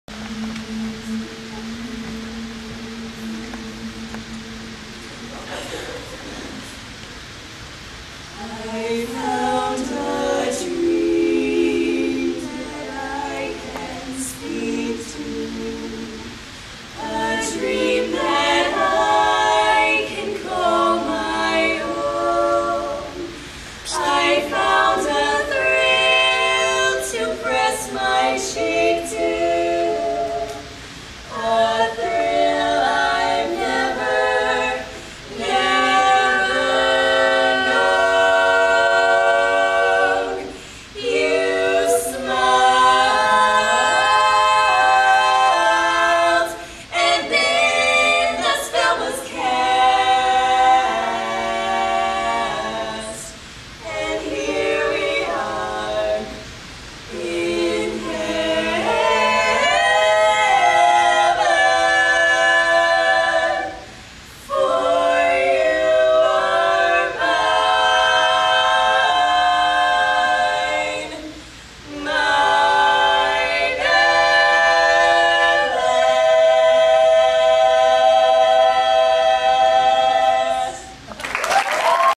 Sam Jobe & Diva @ LHS Vocal Awards 09
Diva performing  at the LHS Vocal Awards 09
Shaelie Schwitters, Veronica Rosa, Megan Niber and Sam Jobe